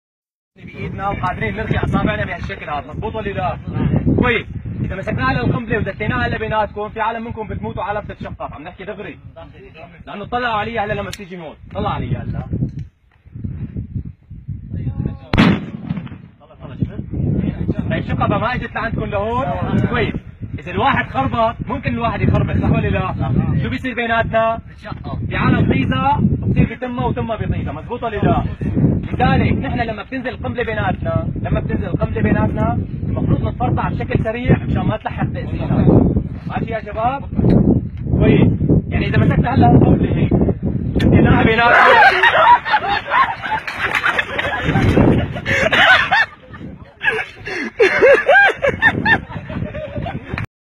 Grenade prank
Officer in Syrian army pranks its colleagues with a grenade